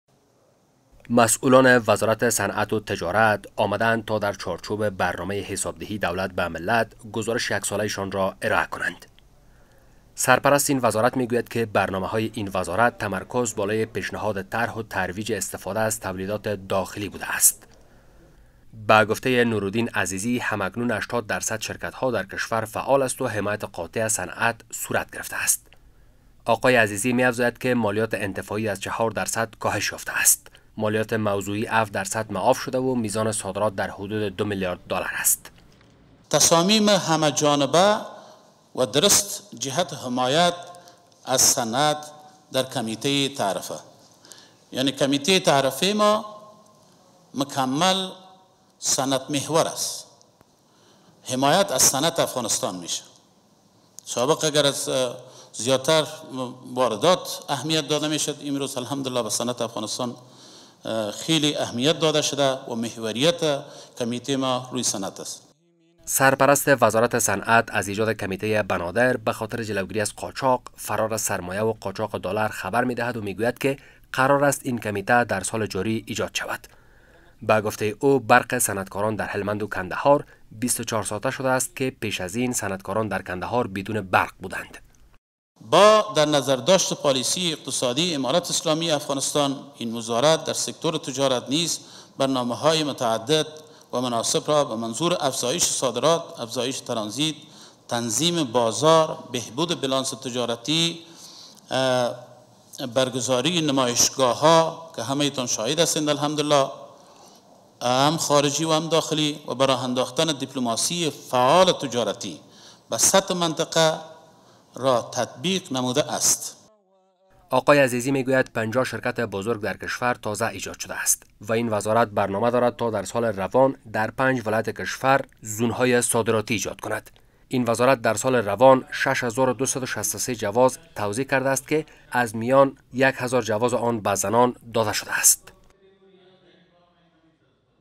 Commerce minister says exports totaled $2 billion dollars in past year
نورالدین عزیزی: میزان صادرات به دو میلیارد دالر امریکایی رسیده است